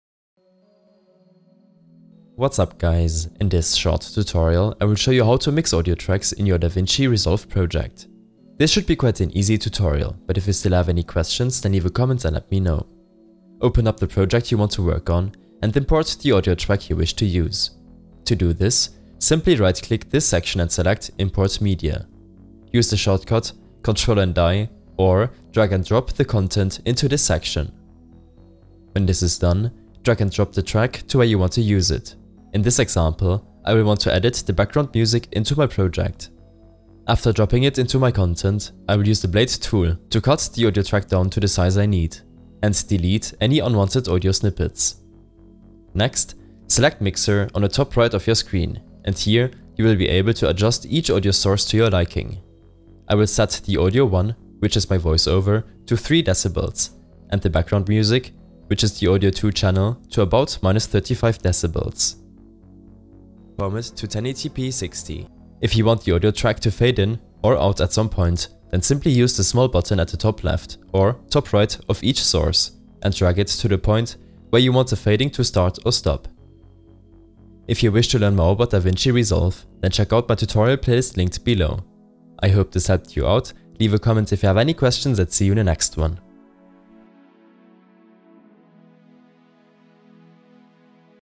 DaVinci Resolve - How To Mix Audio Tracks in your Timeline
A short tutorial on how to mix audio tracks in your timeline in DaVinci Resolve.